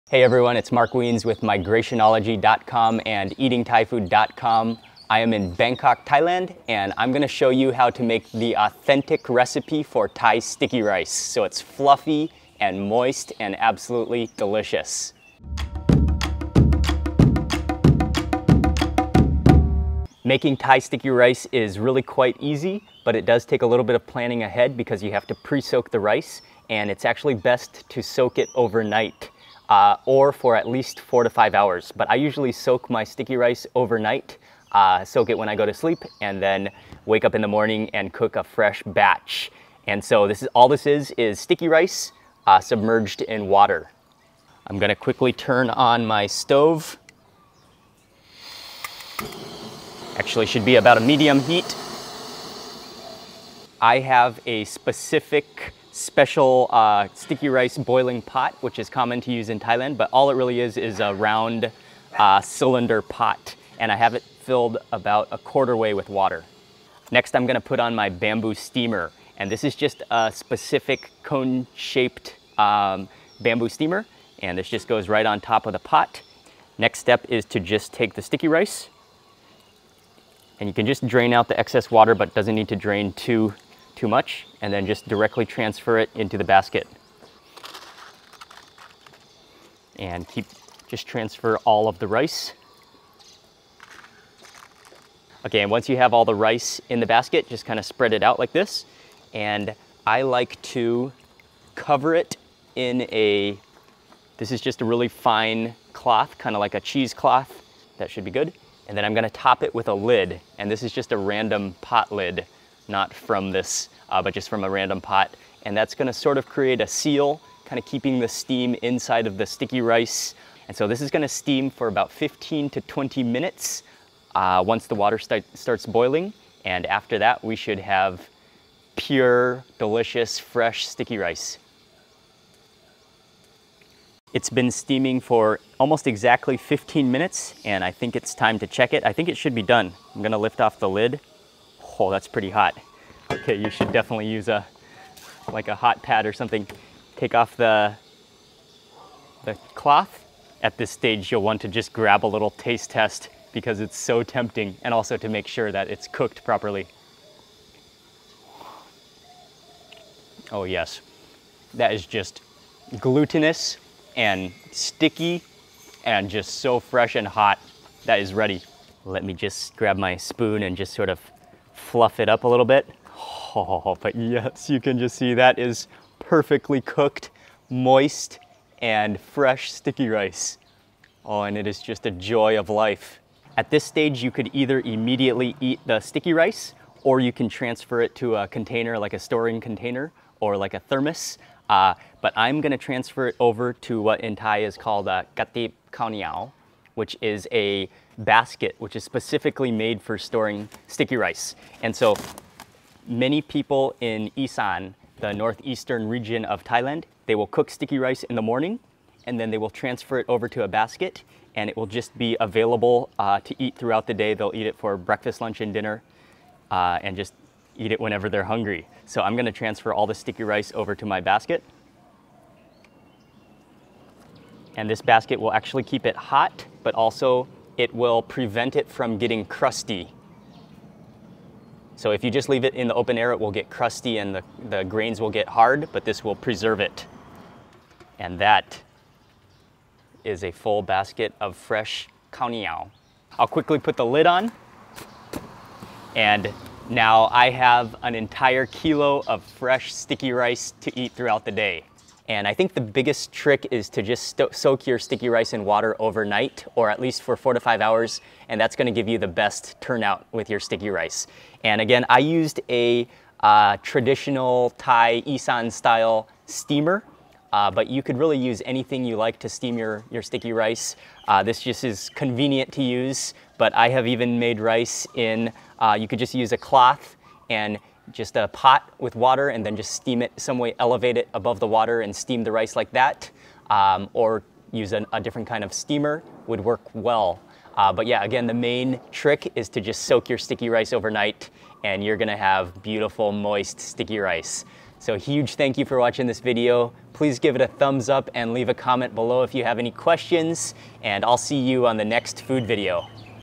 How to make sticky rice (Thai street food style)
Follow this recipe to learn how to make sticky rice the authentic Thai way.

In Thailand, sticky rice, known as khao neow (ข้าวเหนียว) is the staple in the northern and northeastern regions of the country. Many Thais that originate from sticky rice regions, eat sticky rice multiple times a day, for breakfast, lunch, dinner, and even snack. For this sticky rice recipe, I’m going to be making it the same way they make it in Isaan and other parts of Thailand, and I’m going to be cooking it right on my Thai street food cart.

Sticky rice is often eaten with salads and grilled meats, dishes like som tam (green papaya salad), laab (minced meat salad), and all sorts of grilled meats and fish. Along the streets of Thailand, nearly everywhere you look in Bangkok, you’ll find food vendors grilling up skewers of pork called moo ping, and they always (unless they run out) also have sticky rice. The combination is one of the ultimate snacks combinations of Thailand.

In parts of Thailand where sticky rice is the staple, it’s typically cooked in the morning for the entire household, and then transferred to what is called a kratib khao neow (กระติ๊บข้าวเหนียว), a basket that is specifically made to hold sticky rice - it’s just a cylinder bamboo basket with a lid, and it keeps the sticky rice moist without getting crusty throughout the day. When meal times roll around, all participating eaters grab a handful portion of sticky rice and proceed by taking a ball of sticky rice about the size of an olive, dipping it into the dressing of one of the side dishes at the table, and consuming.

How to make sticky rice:

It takes a little time and some thinking ahead to make sticky rice, because just like cooking beans, you have to soak the rice before you cook it. It’s best to actually soak your sticky rice overnight, and then cook it the next day, but if you don’t have that amount of time, of if you wake up one morning and all of a sudden decide you want to cook sticky rice, you should soak it for at least four to five hours. All you have to do is put the raw sticky rice is a bowl or tub, and submerge it in room temperature water, and allow it to sit. You don’t even need to rinse the rice, just let it sit and soak. After the rice has been soaked, it will become a little swollen and much softer, so that it won’t take too long to steam.

After soaking the rice, I then transferred it to a cone shaped basket used for steaming. The rice then needs to steam in a pot with a few inches of water at the bottom for 15 to 20 minutes, but usually it just takes 15 minutes for me - that is if I soak the rice overnight. If you soak the rice for less time, it may take a little longer to steam. To steam, make sure the rice is elevated above the boiling water, then cover it somehow so the steam remains within the rice.

After 15 minutes you can remove the lid, and check to see if the rice is fluffy and not crunchy at all. If it’s still a little crunchy, just put the lid back on the basket, or whatever steamer you’re using, and allow to steam for a few more minutes.

For this Thai sticky rice recipe I used one kilo of raw rice, but you can really use however much you want, and the exact same directs and steps will still apply. If you soak your sticky rice overnight, then follow this recipe, you’ll have beautiful, hot and fluffy sticky rice every time… and it tastes amazing!

Be sure to check out the full recipe and lots more details

Thank you very much for watching this recipe, and see you on the next food video.